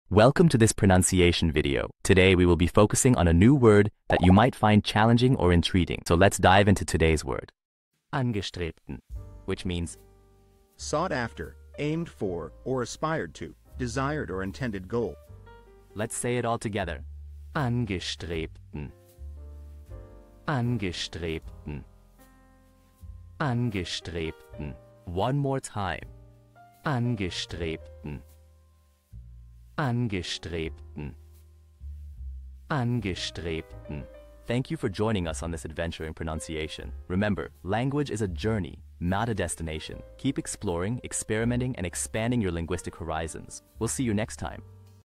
How to pronounce Angestrebten
Master the Pronunciation of 'Angestrebten Which means desired' - which means : Sough after, aimed for, or aspired to;desired or intended goal. 📝 with @PronunciationGerman 🎓🗣️ - [Your Guide to German]

Learn to pronounce 'Angestrebten' perfectly in German with our easy and engaging tutorial by @PronunciationGerman! 📖📽️ In this comprehensive video, we provide you with native speaker examples 🗣️👂 and phonetic breakdowns 🎵 to ensure you get the pronunciation just right.

How to say Desired in German